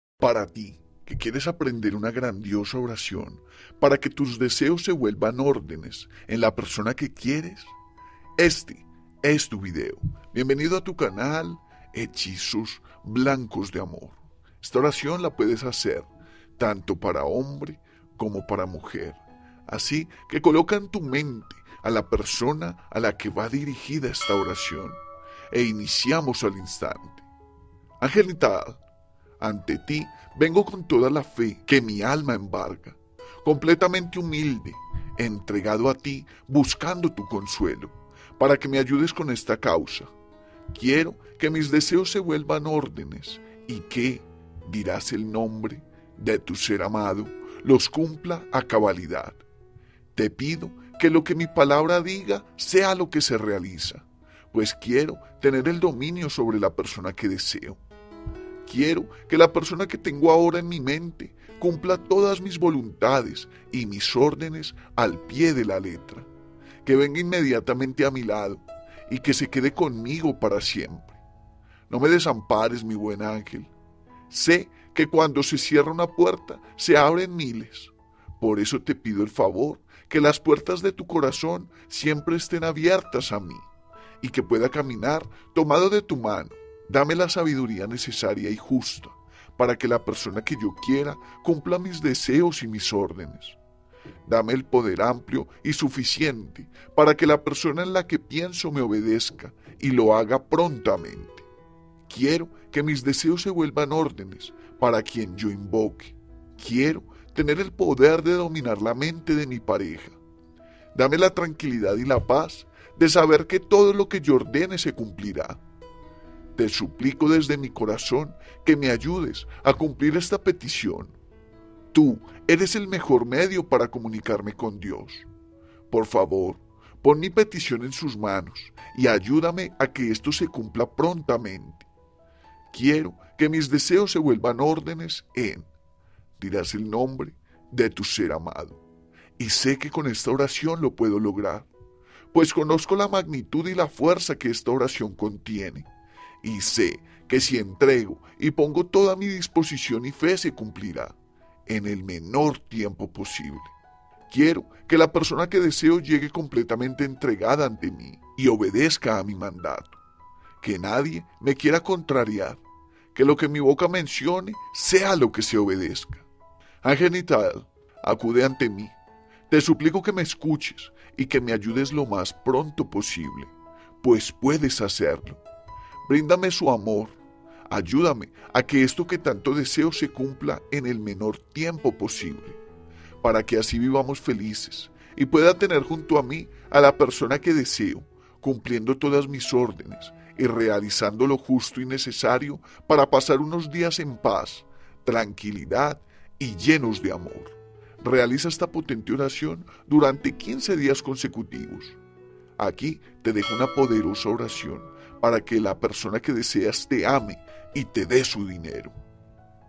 PARA QUE TUS DESEOS SE VUELVAN ORDENES Oración fuerte para DOMINAR el amor
Haz esta oracion para hacer realidad tus deseos de amor
Escucha esta potente oracion para hacer realidad tus deseos de amor